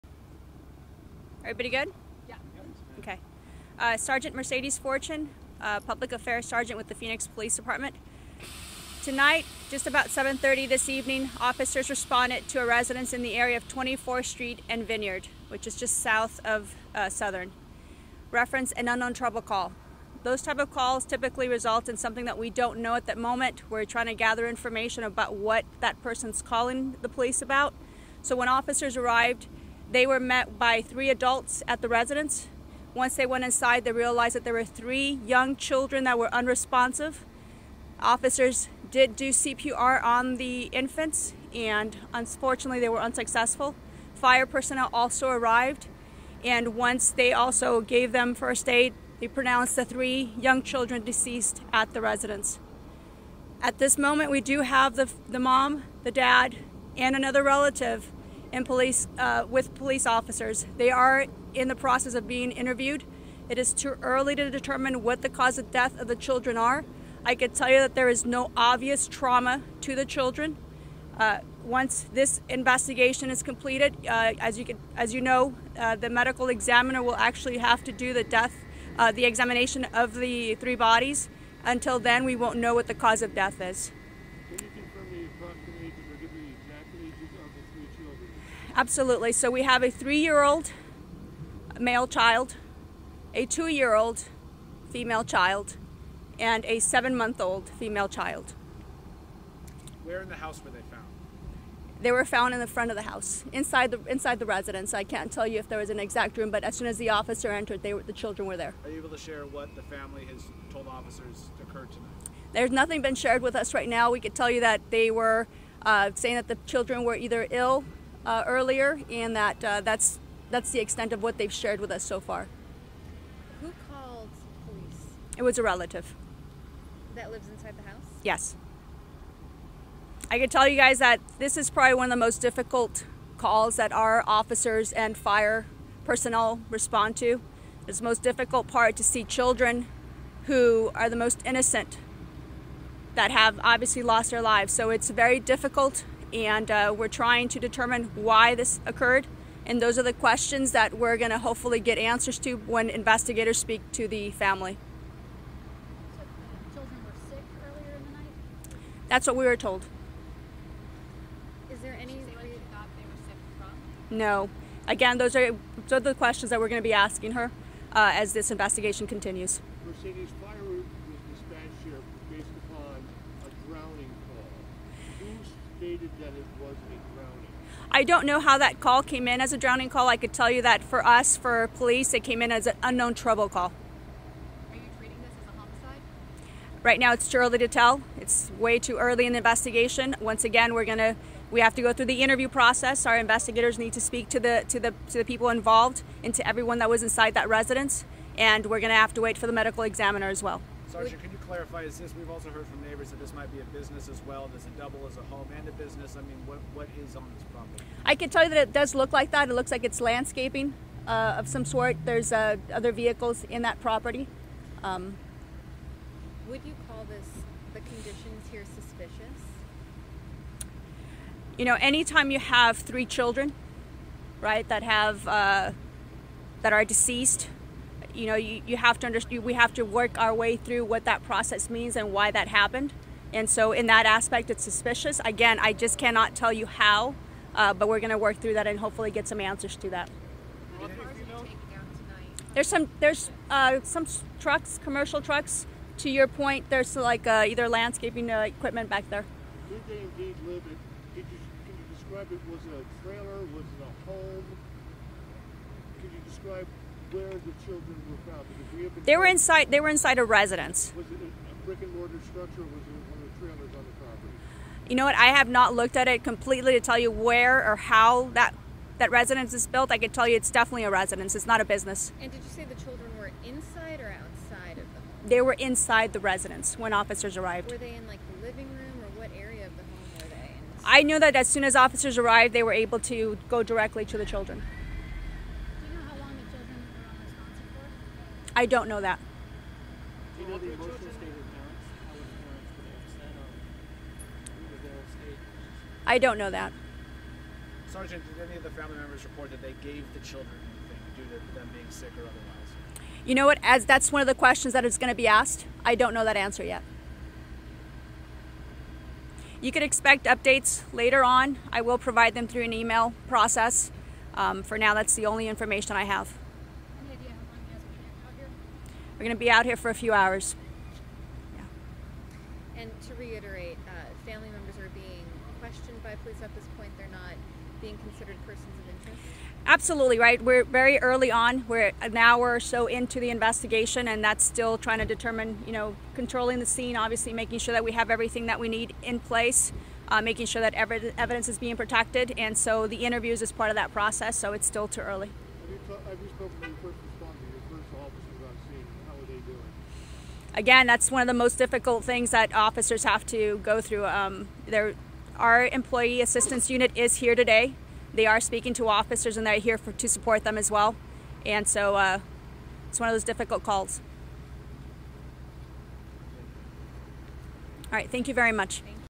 Police hold news conference about 3 young children found dead in Phoenix
It is too early to confirm the cause of death, Sgt. Mercedes Fortune of the Phoenix Police Department said.